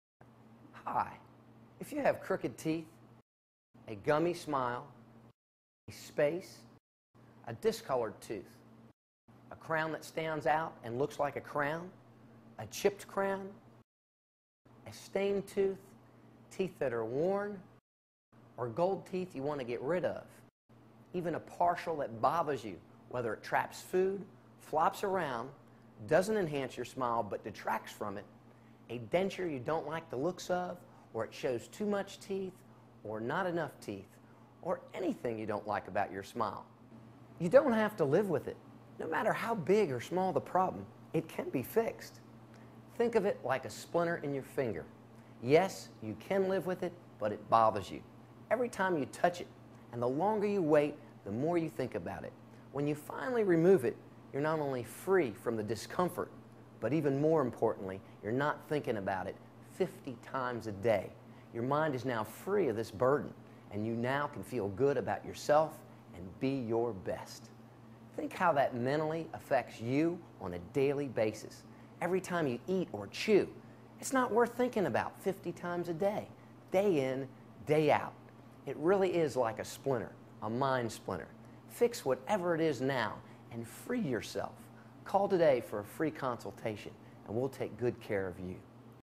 Dr G Like A Splinter
Dr. G is one of the top cosmetic dentists in the United States.  He has a passion for improving patient's lives through smiles.  Here he explains how living with a dental problem is just like having a splinter